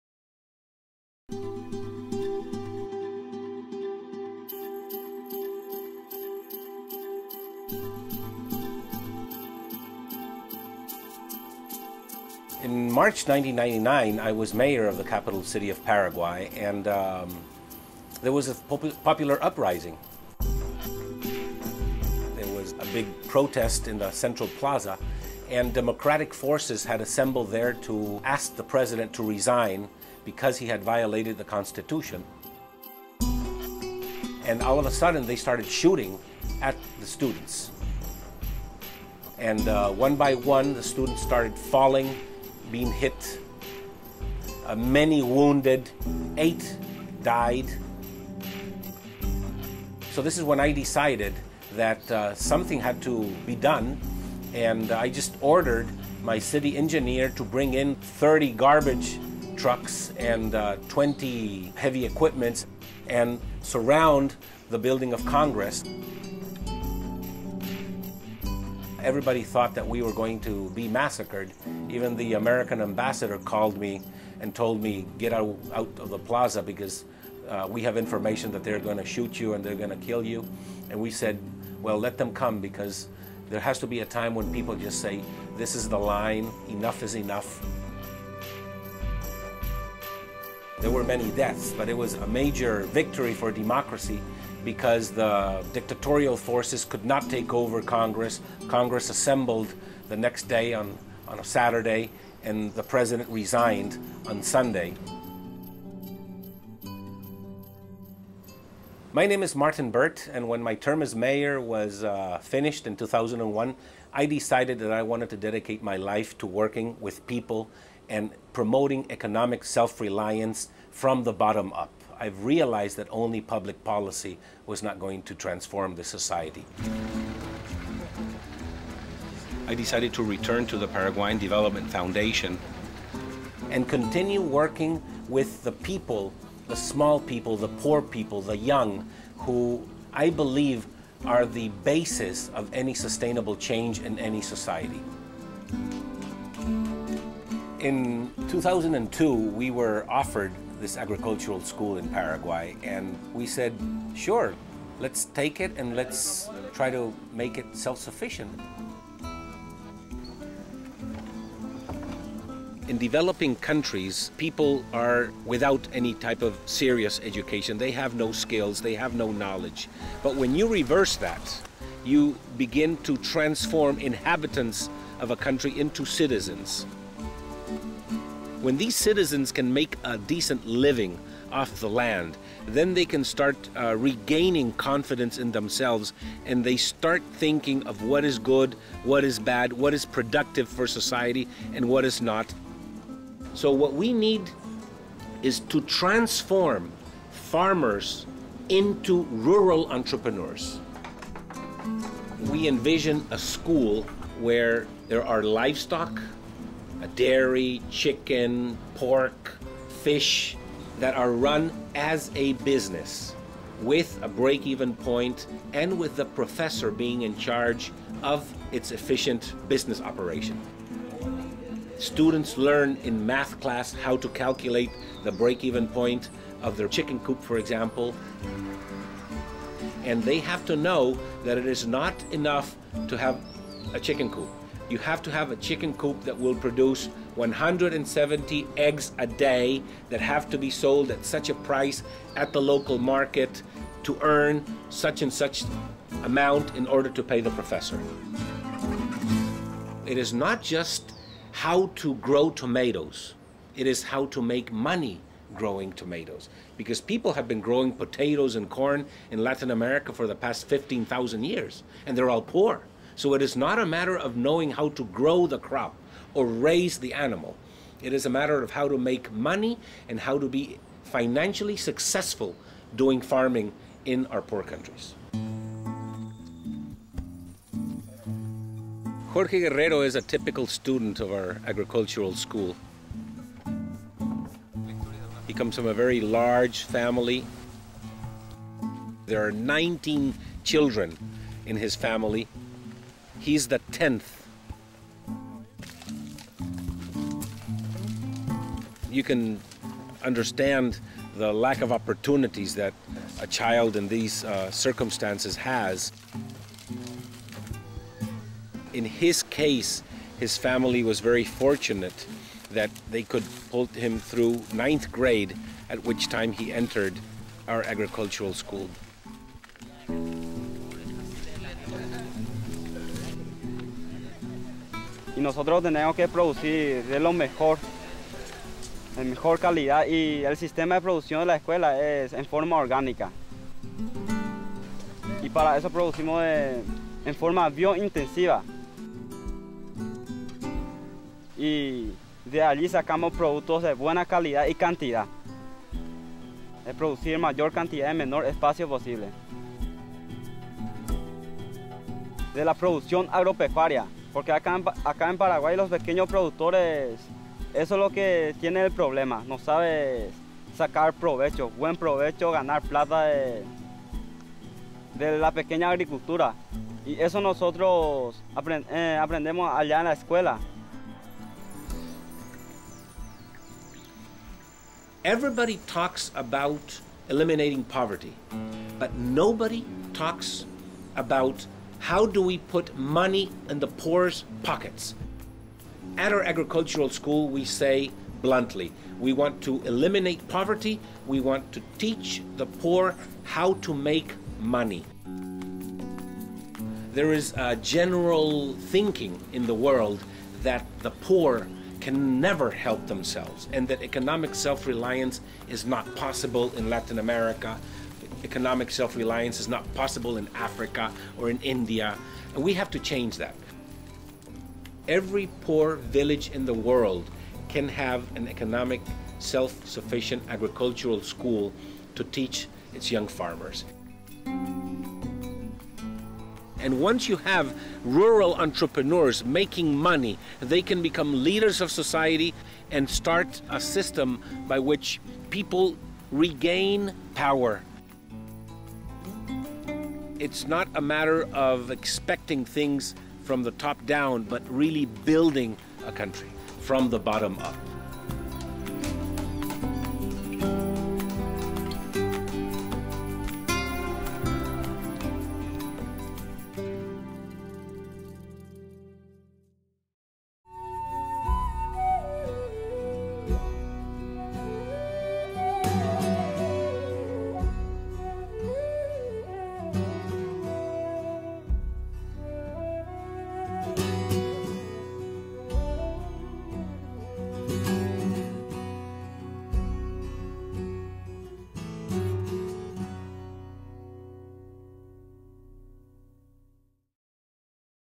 Martin Burt - Uncommon Heroes
Meet Martin Burt. After studying in Spain and the United States, Martin Burt returned to Paraguay in 1985 and started an innovative microcredit program.